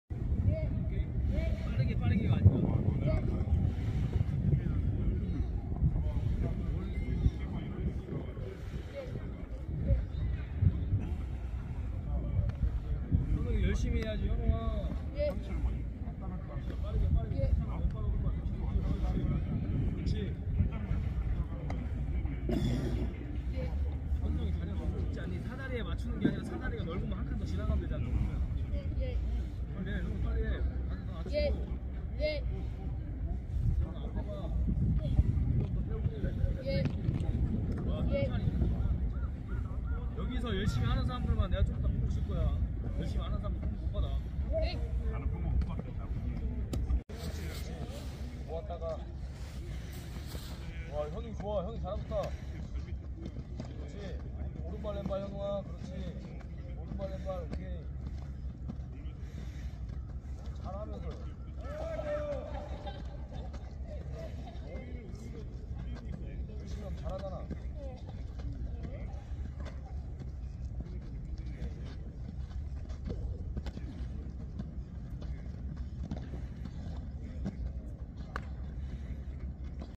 평택시 고덕리틀야구단 선수들 내야 수비 기본기 훈련 영상 x 한국리틀야구연맹
한국리틀야구 동계훈련 현장체크 x 더 베이스볼 팩토리